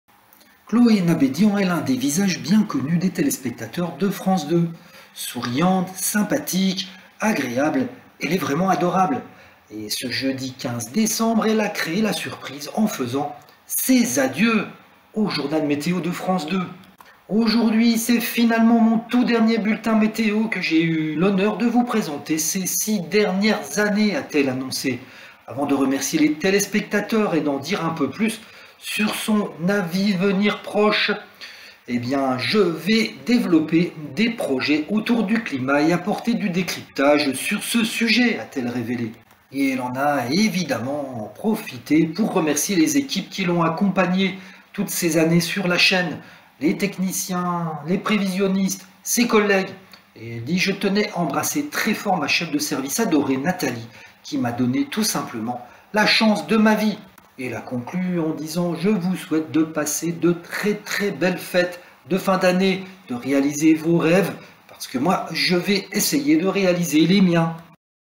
Chloé Nabédian, la gorge serrée : elle annonce une nouvelle incroyable
Chloé Nabédian a fait ses adieux à la météo de France 2, ce jeudi 15 décembre, après six ans de présentation. C'est la gorge serrée qu'elle en a dit un peu plus aux téléspectateurs sur ses projets futurs